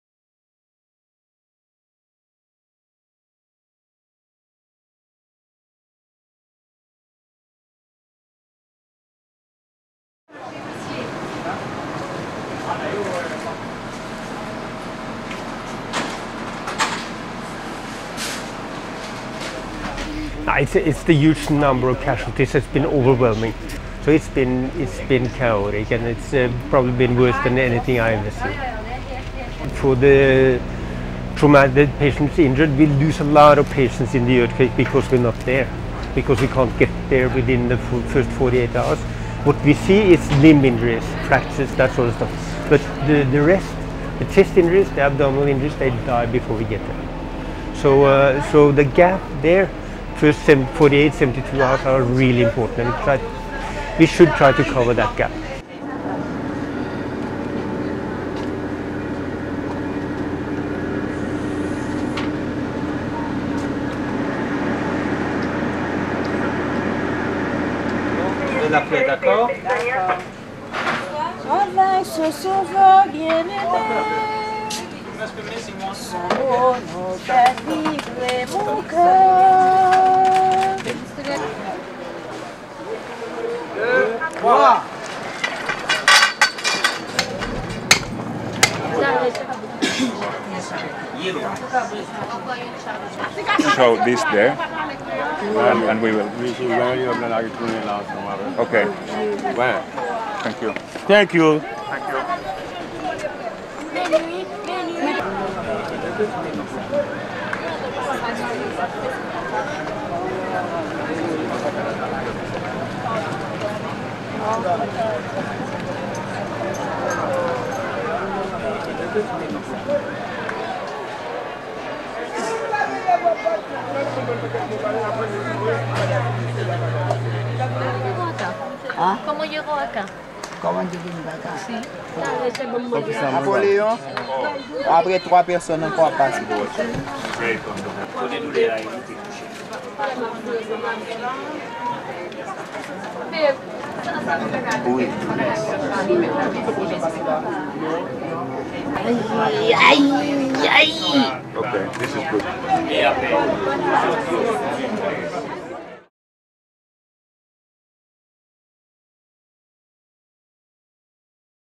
Haiti: medical aid for the survivors
The need for urgent medical aid for earthquake survivors was desperate. Health care personnel from all over the world joined forces to treat the wounded. Field hospitals and operating theatres were set up for the most severe cases. Shortly after the disaster struck, first-aid posts were set up by the Haitian Red Cross and the ICRC, providing basic health care to tens of thousands living in makeshift camps.